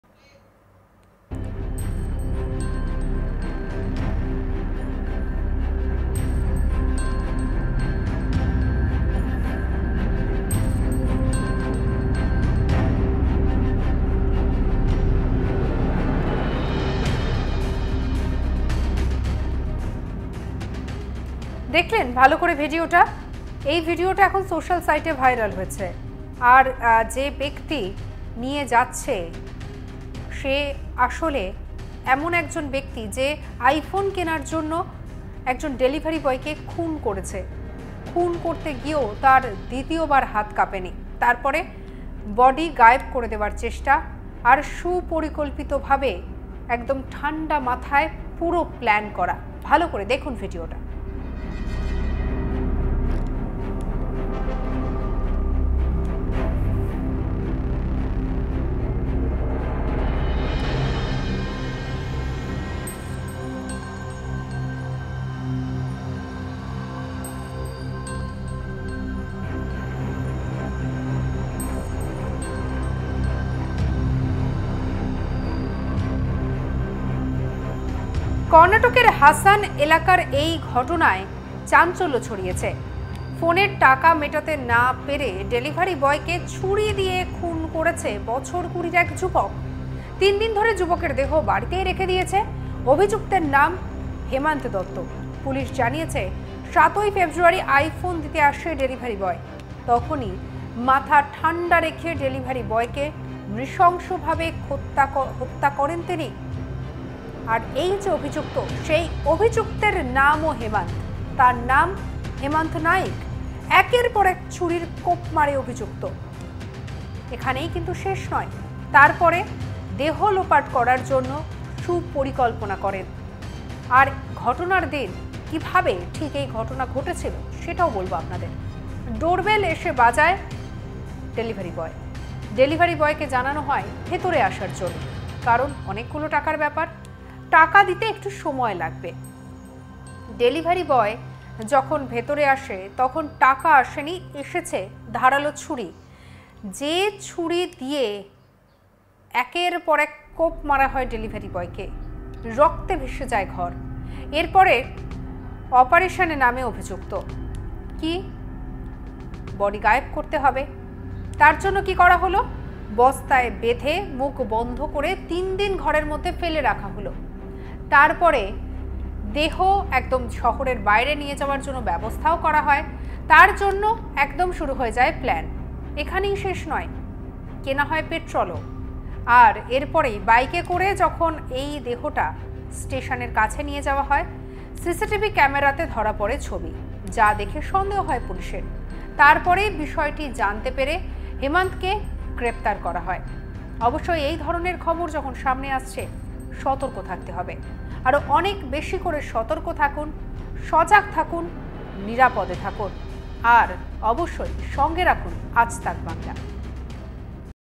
​আইফোনের দাবি! ডেলিভারি বয়কেই খুন | Karnataka | Delivery Boy Murder Case | Aaj Tak Bangla
Unable to pay Rs 46,000 for iPhone he ordered, Karnataka man kills delivery boy